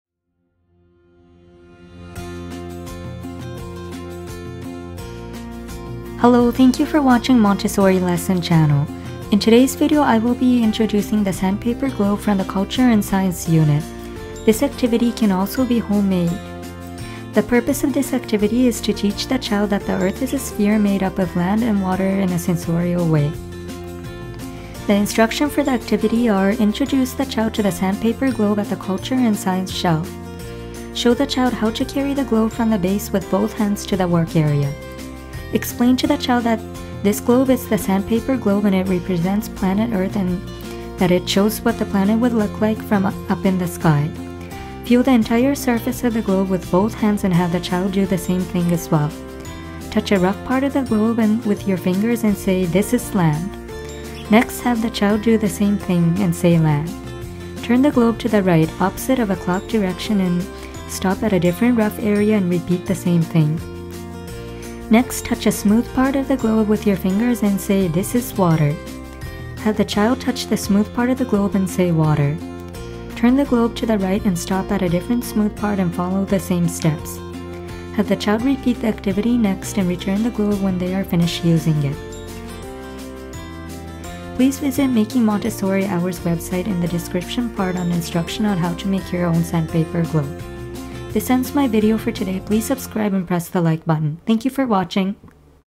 Montessori Culture and Science-Sandpaper Globe lesson
In this video I will be introducing the Sandpaper Globe from the culture and science unit.
Please see the link for Making Montessori Ours website for instructions
on how to make the Sandpaper Globe.